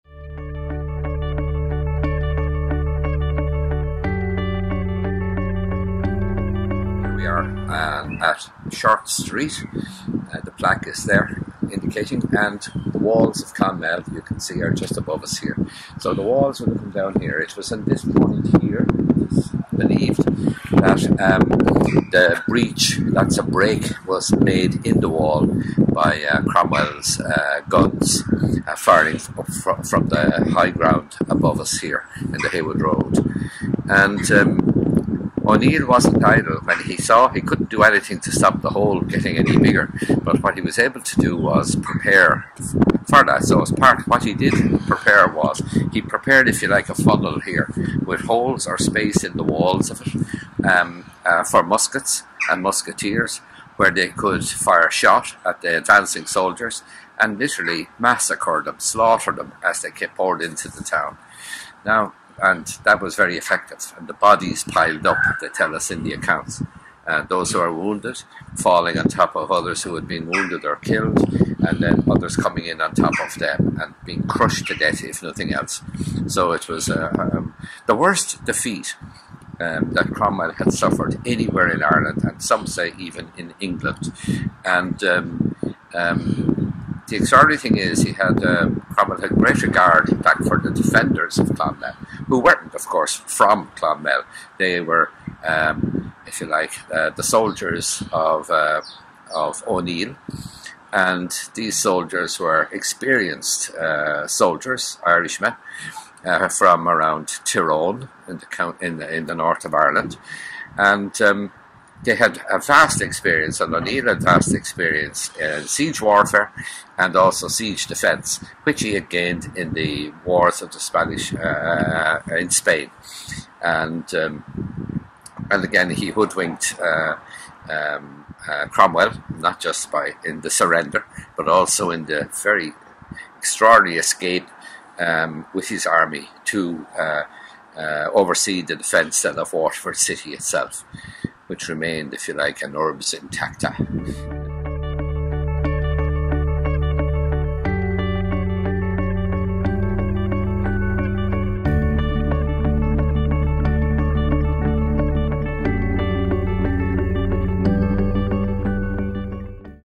Around Our Town Ep. 8 - The Siege of Clonmel
The Siege of Clonmel in 1650 by Oliver Cromwell is one of the most well known historical events in the history of Clonmel. Today Shay visits Short Street where the breech of the town walls occurred and where so many men lost their lives.

Around Our Town is a project being undertaken by Clonmel Library and local Clonmel man Shay Hurley. Over the coming weeks we will take a closer look at some of the town’s historical places of interest.